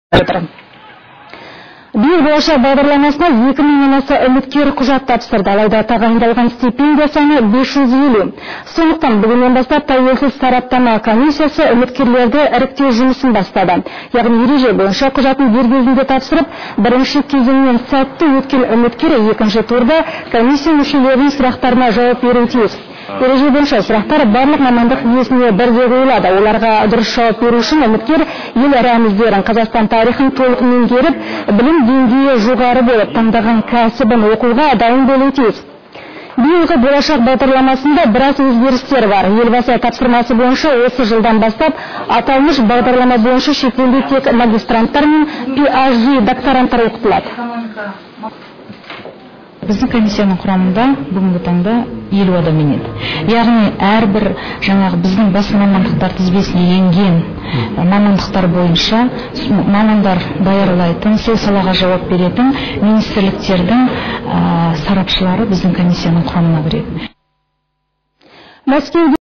«Болашақ» бағдарламасына 2000 құжат қабылданды
Биыл «Болашақ» бағдарламасына 2000-нан аса үміткер құжат тапсырды. Алайда тағайындалған стипендия саны 550.